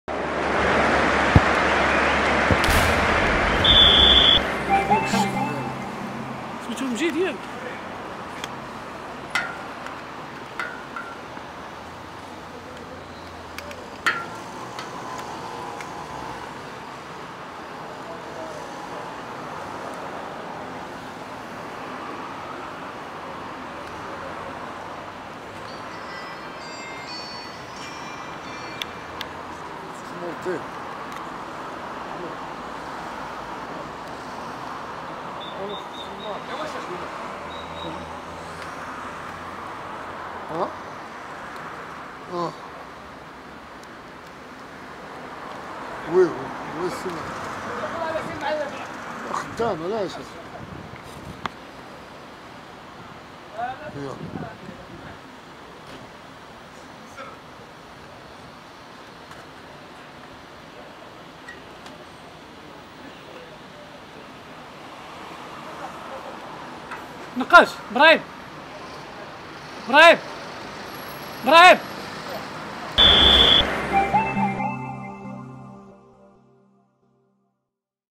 بعد إنتهاء المباراة .. النقاش يتوجه لحافلة الوداد و يُجري "دردشة قصيرة" مع المياغري و بنشريفة
البطولة | بعد إنتهاء المباراة .. النقاش يتوجه لحافلة الوداد و يُجري "دردشة قصيرة" مع المياغري و بنشريفة